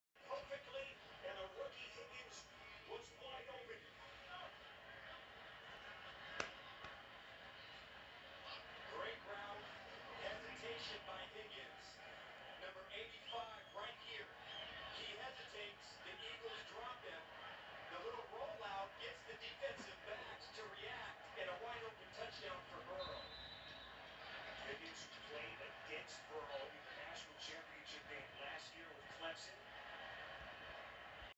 Joe burrows TD pass to tee Higgins
My first time seeing Joe burrows throw a touchdown pass